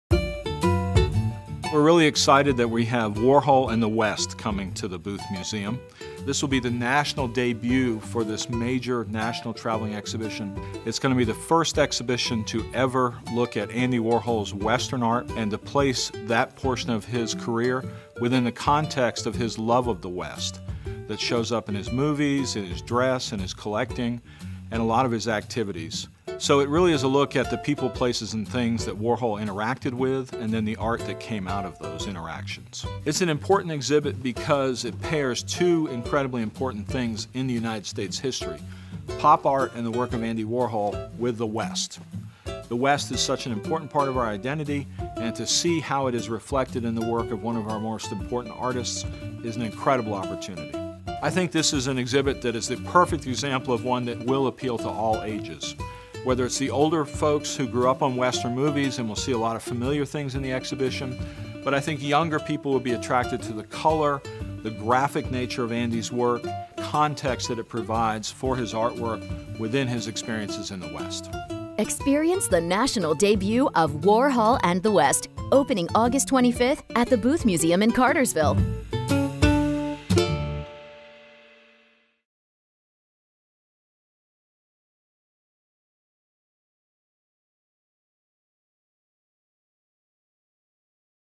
'Warhol and the West': Traveling exhibition opening at Booth Western Art Museum
'Warhol and the West' is the first ever museum show to explore the full range of Western imagery Warhol produced and how his love for Western movies and the region shaped his ironic artistic commentary on how Americans mythologize the West. Co-developed with the National Cowboy & Western Heritage Museum and the Tacoma Art Museum, 'Warhol and the West' is making its national debut at the Booth (August 25 – December 31, 2019) before moving on to Oklahoma City and Tacoma, Washington in 2020. Exhibition catalogue co-published by University of California Press in association with the Tacoma Art Museum.